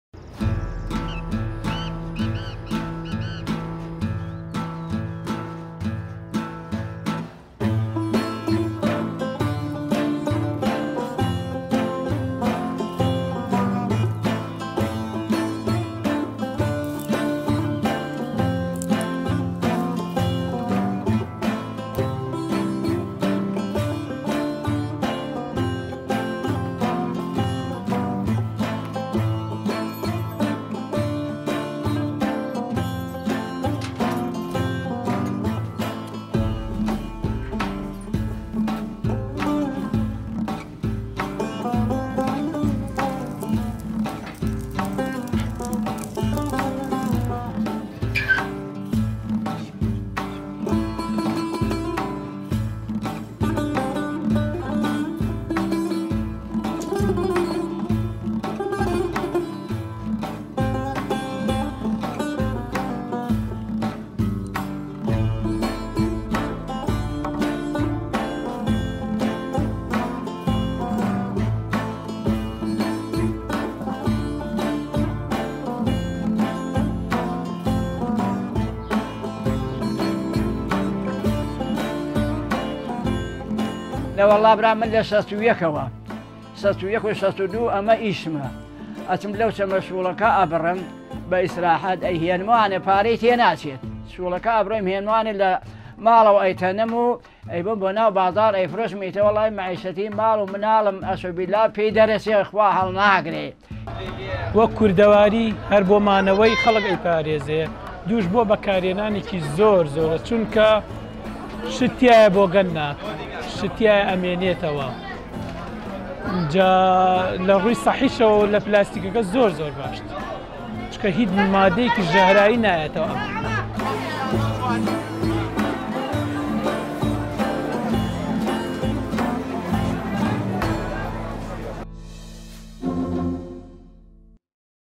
لحفظ الخبز وتغطية الطعام دون ترك السموم كما يفعل البلاستيك.. تعرف على صناعة “الطبك”
أمام موقد الحطب وعلى أنغام أغانٍ كردية قديمة، يمارس غريب عبدالله مهنة صناعة السلال، أو كما يطلق عليها “الطبك“، التي ورثها عن والده في صغره، في كل صباح يتجه عبدالله لجمع الأغصان والعيدان قرب جدول ماء قرية كن كاوه في قضاء شهرزور في محافظة حلبجة العراقية، ليعود ويصنع السلال ليجهزها للبيع في السوق، باعتبارها مصدر الدخل الوحيد له ولعائلته، يذكر أن الطبك يستعمل لحفظ الخبز، وتغطية الطعام كونه لا يترك السموم في الطعام كما يفعل البلاستيك.